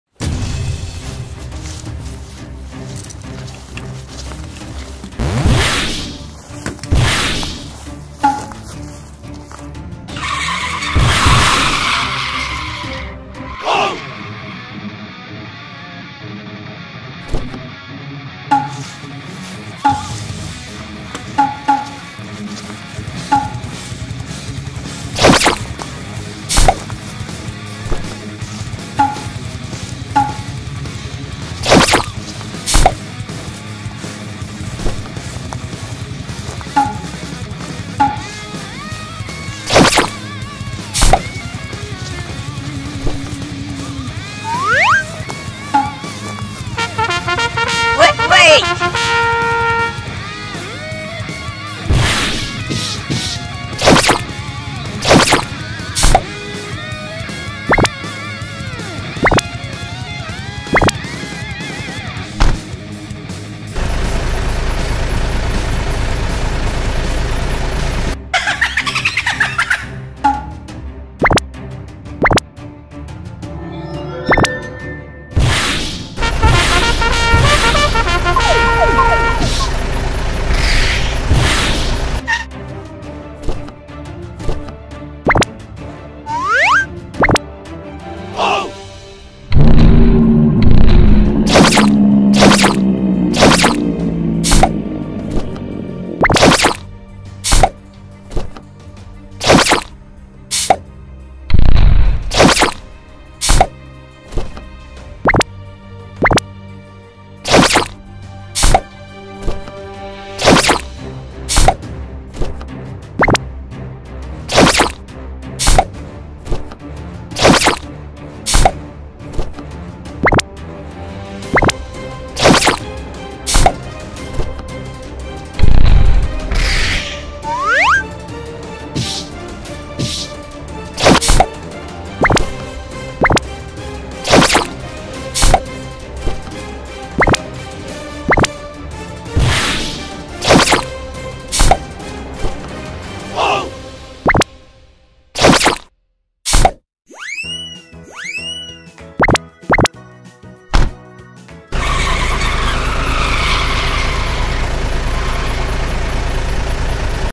Giant Monster Insects Go into the Garbage truck toy Green Power Ranger drives はたらくくるま ゴミ収集車 すぽすぽタイム
Giant Insects-Centipede,Spider,Cockroach- Go into the Garbage truck.Green and Red Power Rangers help Minion
Let's Enjoy Super Spo Spo Time

ゴキブリとムカデがミニオンズを・・・
そして赤い戦隊ヒーローと緑のパワーレンジャーが救出